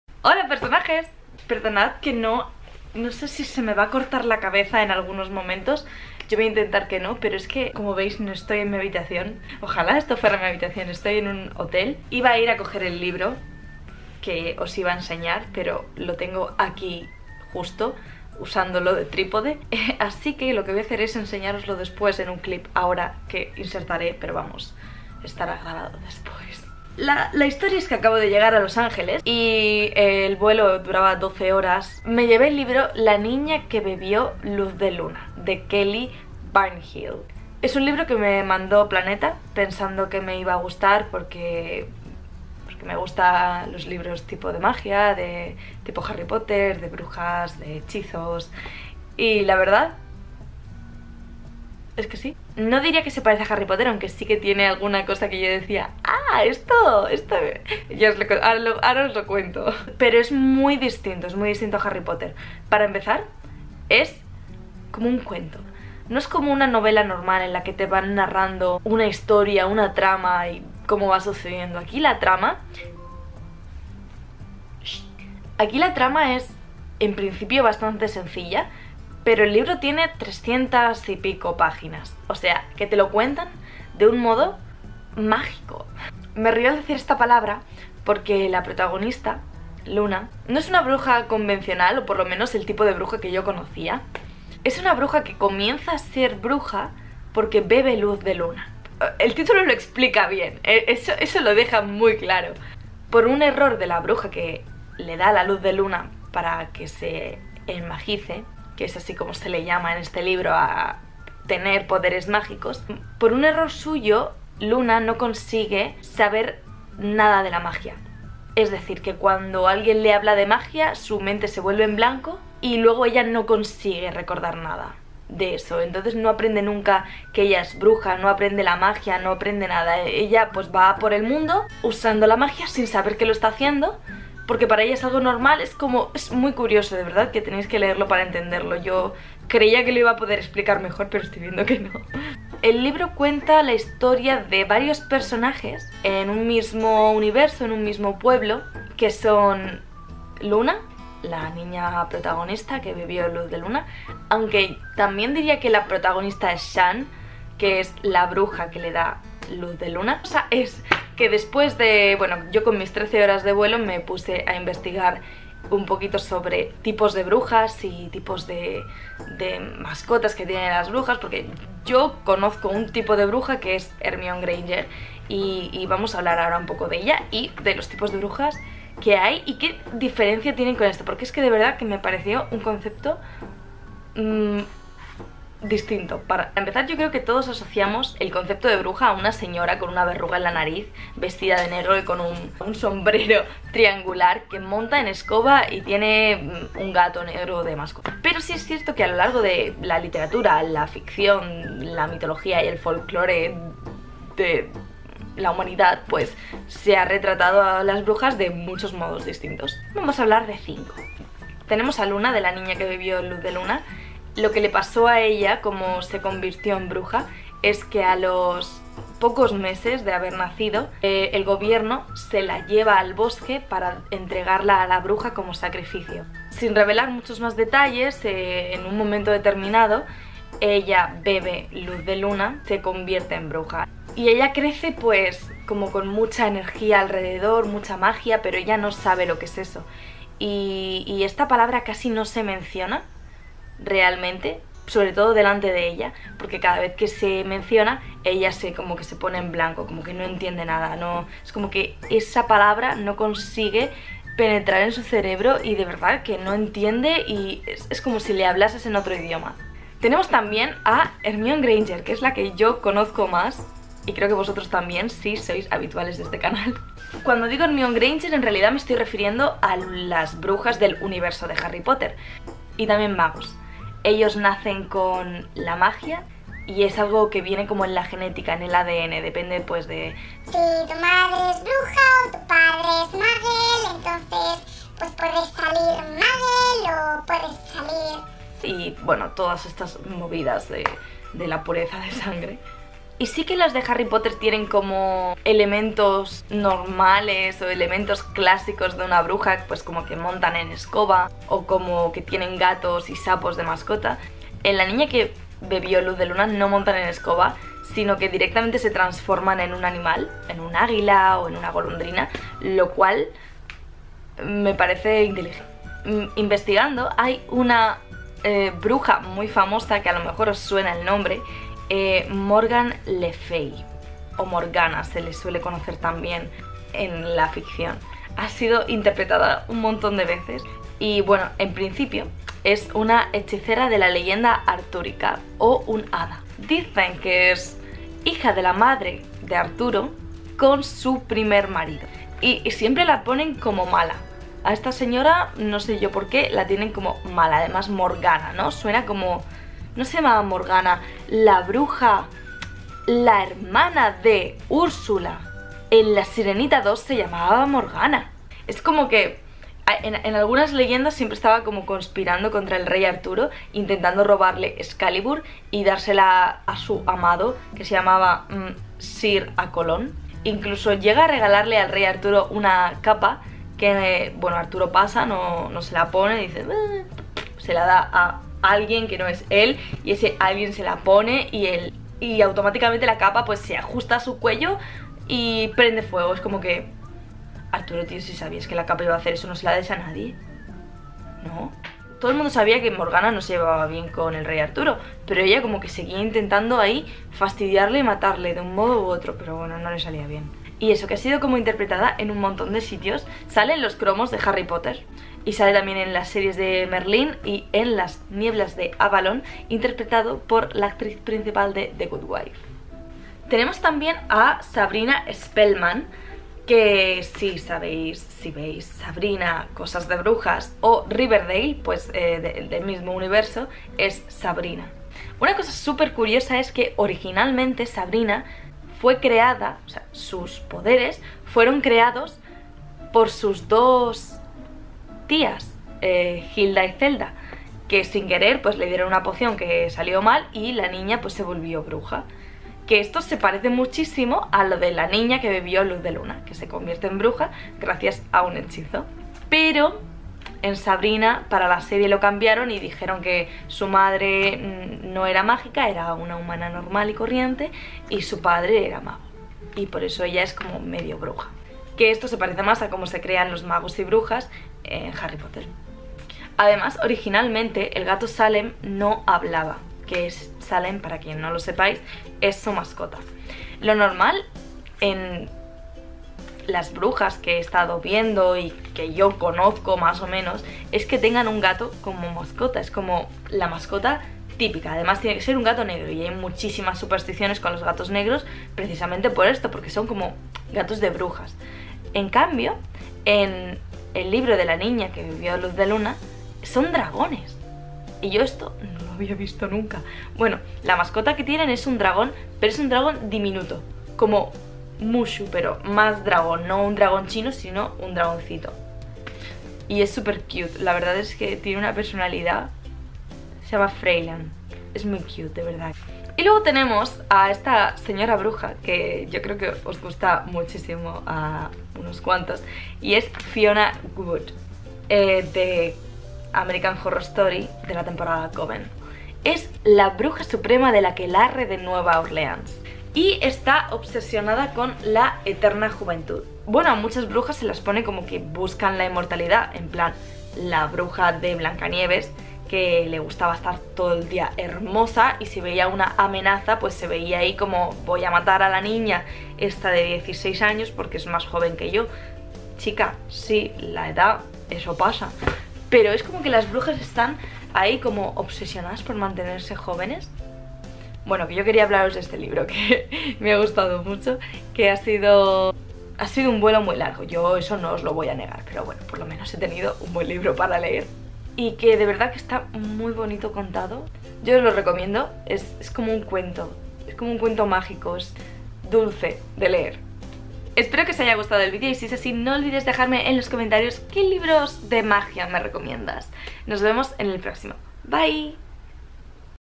5 TIPOS DE BRUJAS EN FICCIÓN | Porexpan
¿Cuál es tu bruja favorita de libros, películas y comics?

Esta es el libro del que os hablo:
"La niña que bebió luz de luna"

Este libro está patrocinado por Planeta, pero todas las opiniones son mías y verdaderas.